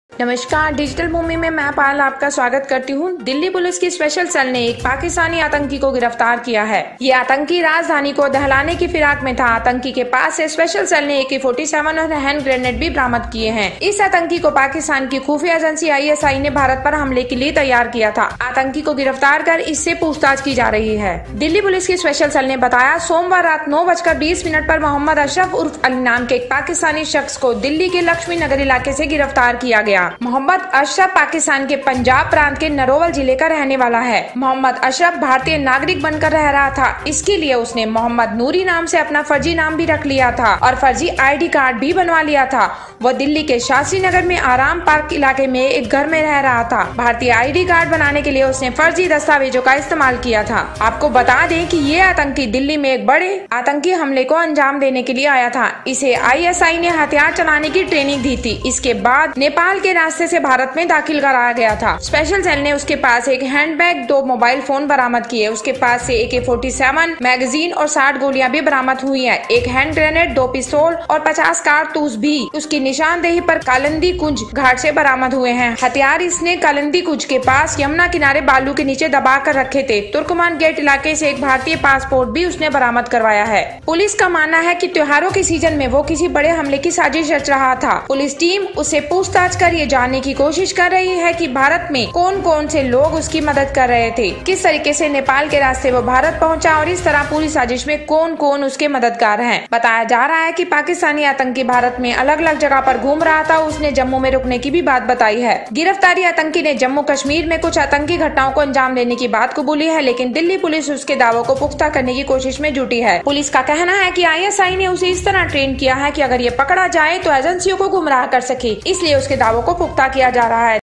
Delhi से पाकिस्तानी आतंकी एके-47, हैंड ग्रेनेड के साथ गिरफ्तार, कई शहरों को दहलाने का था ईरादा
Pakistani terrorist arrested with hand grenade and AK-47 from Delhi, intended to shake many cities